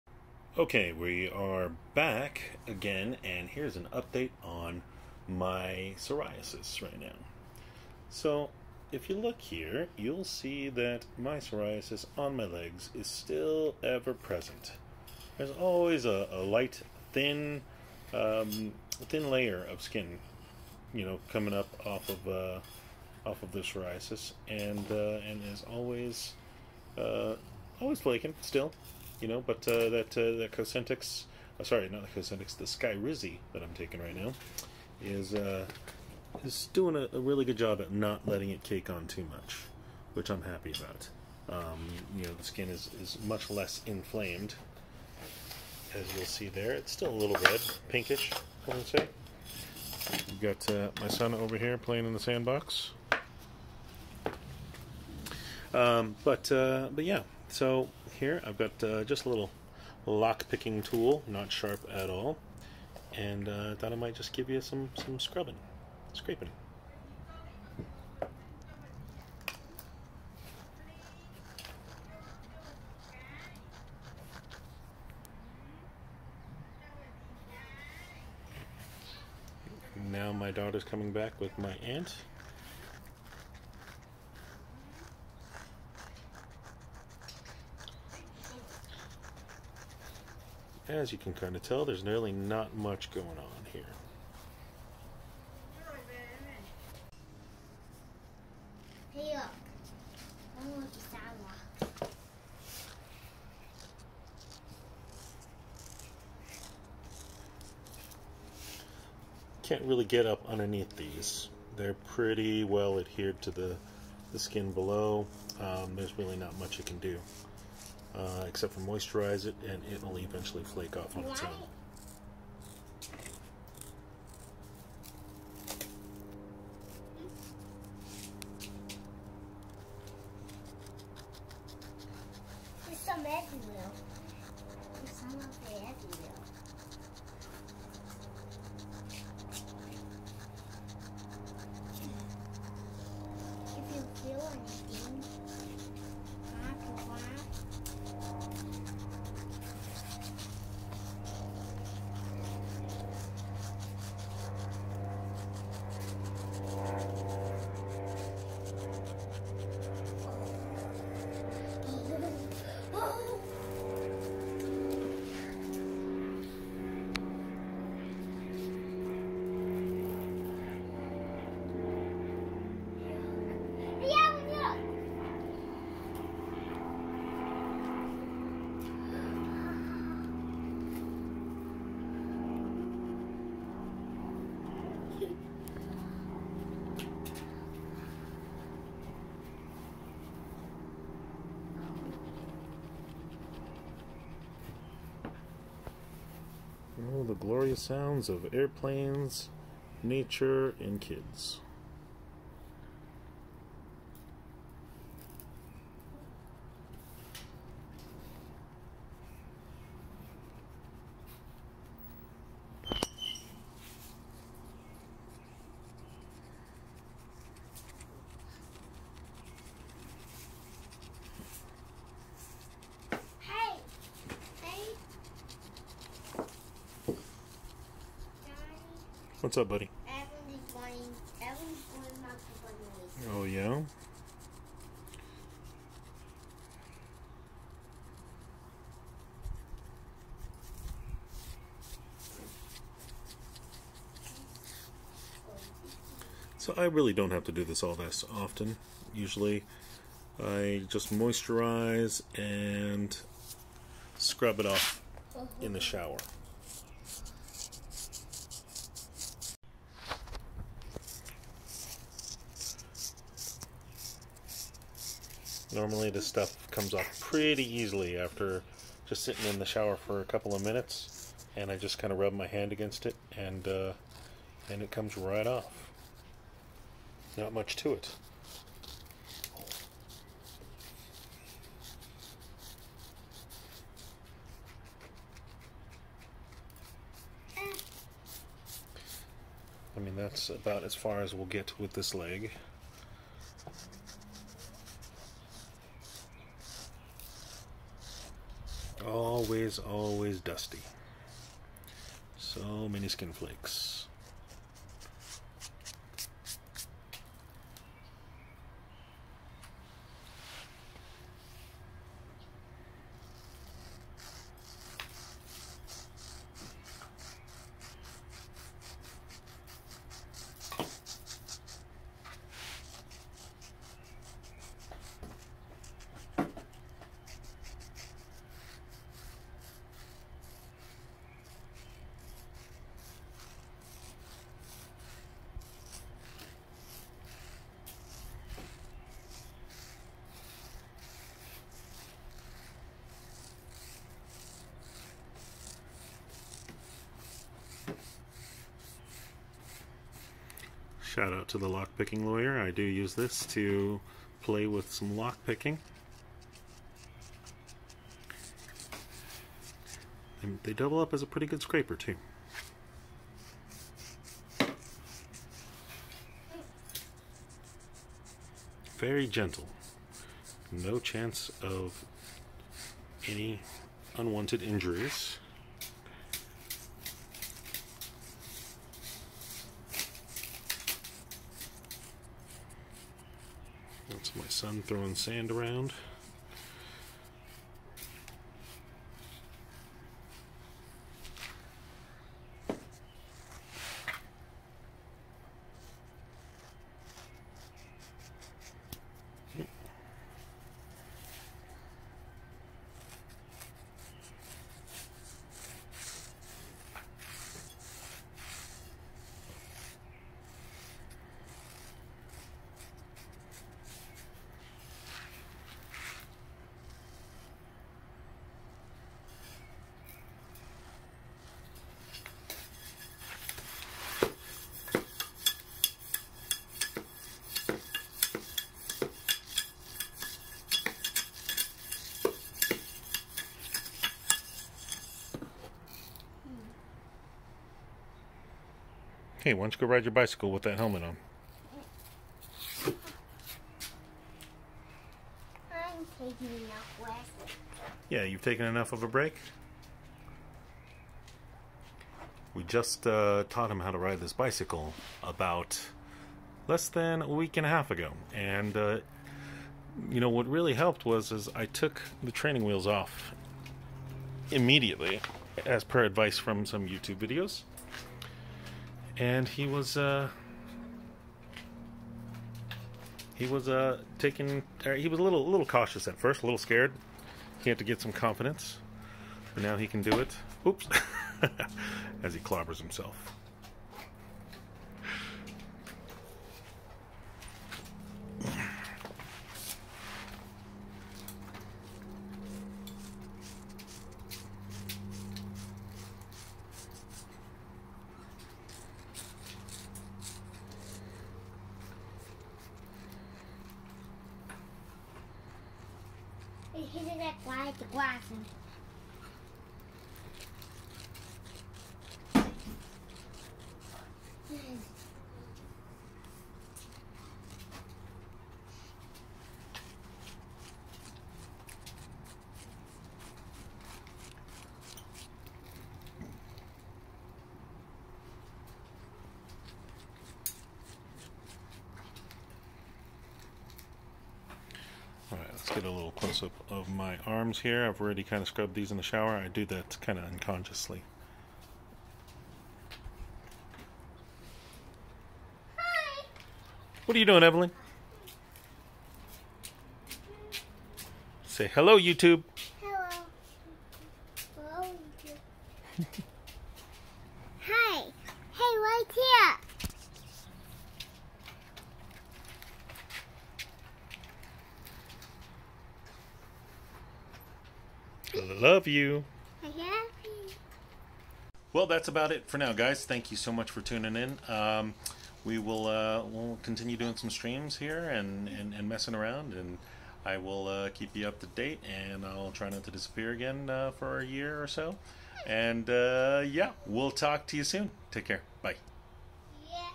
Why Don't You Come Up and Scrape Me Some Time... [Psoriasis]
Update and scraping with a LOCKPICK?

I am an experienced Psoriasis sufferer and I choose to exfoliate the scales in my own ways to cope with my condition.  Please consult a doctor about your skin questions.

🥥 Favorite Moisturizers?

There are many types of psoriasis:
plaque
guttate
inverse
pustular
erythrodermic
psoriatic arthritis